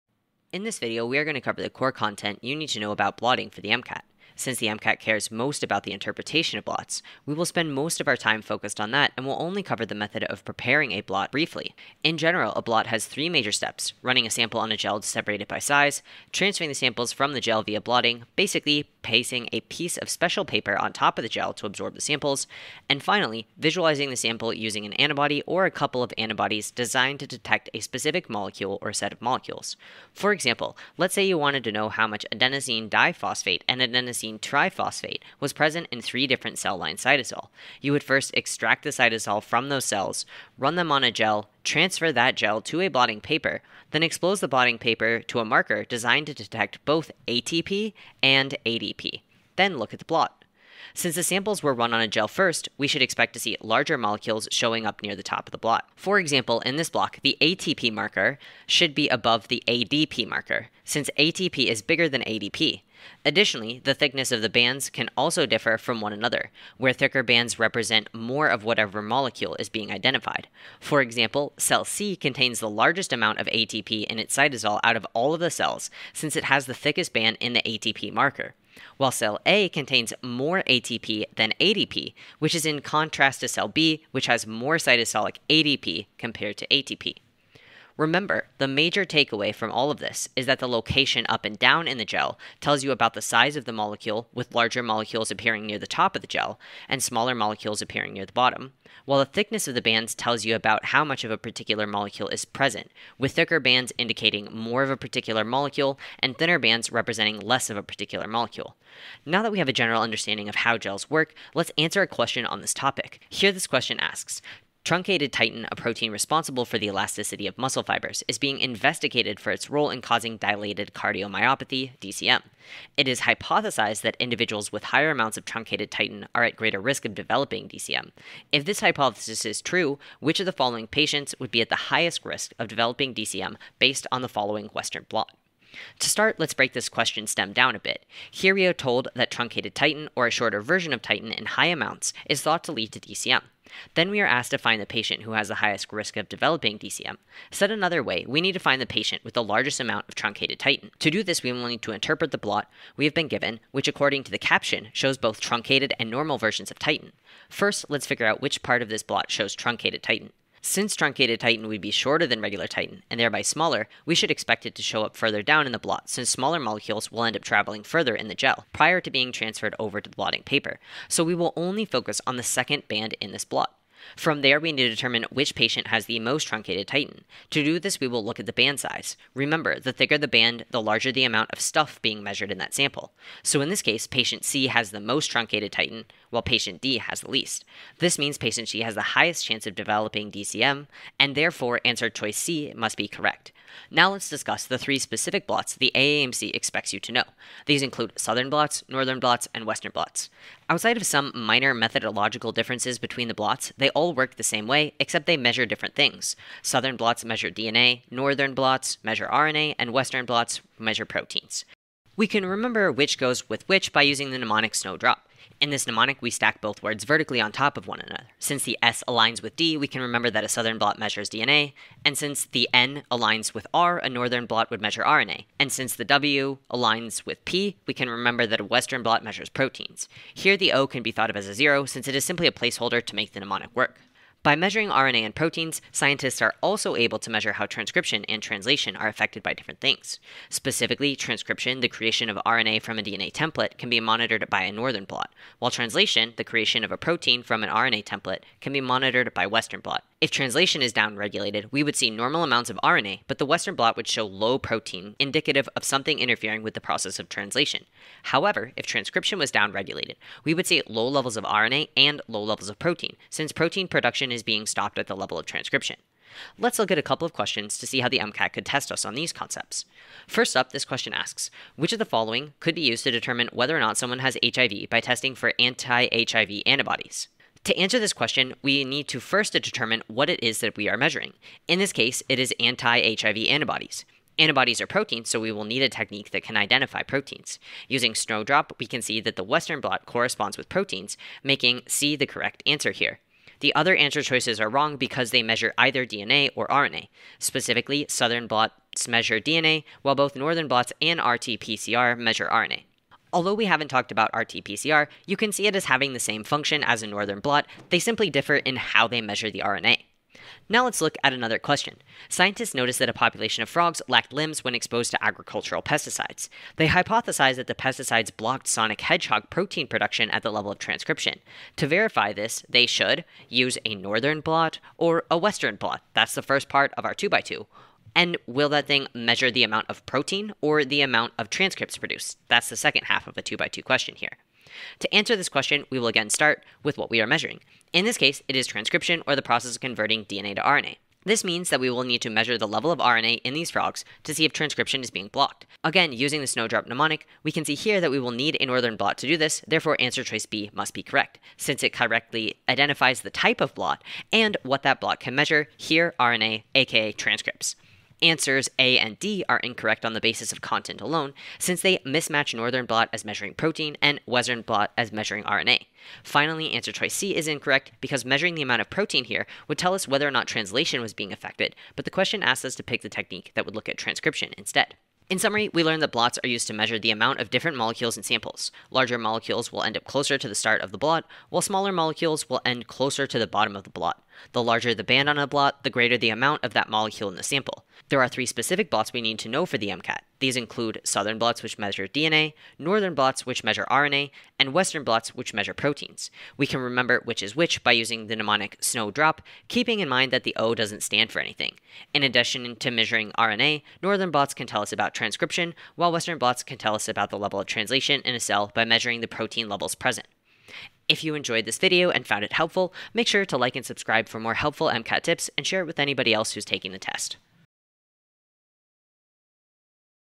Blotting: How It Works and The Different Types | MCAT Content
In this video, we will cover blotting for the MCAT. This includes an overview of the general technique, but more importantly how to interpret blots. From there we will explore the three different blots the MCAT wants you to know including Southern Blots, Northern Blots, and Western Blots.

As always we will round out our discussion of blots with a couple of practice problems so you walk away with an understanding of how to apply what you have learned.

00:00 - Intro
00:12 - How Blots Work
00:31 - How To Interpret Blots
01:48 - General Blot Practice Problems
03:14 - Southern, Northern, and Western Blots
03:57 - Blots and Central Dogma
04:25 - SNOW DROP Problems
06:14 - Blot Recap